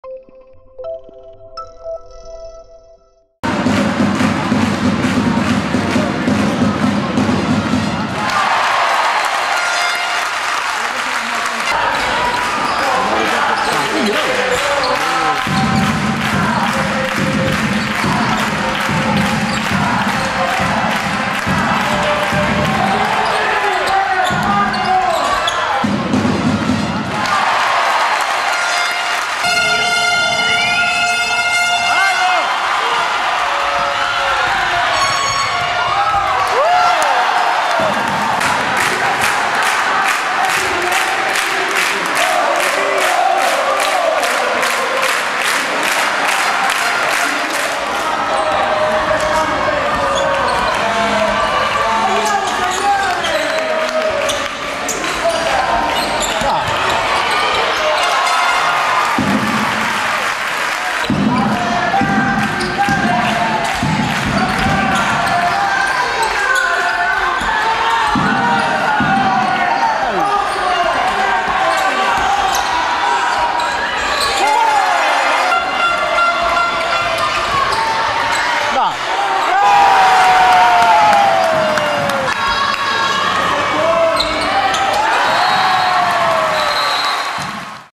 Due Esse Basket VS Rieti Basket Club
Gara Tre dei playoff contro il Rieti al PalaWojtyla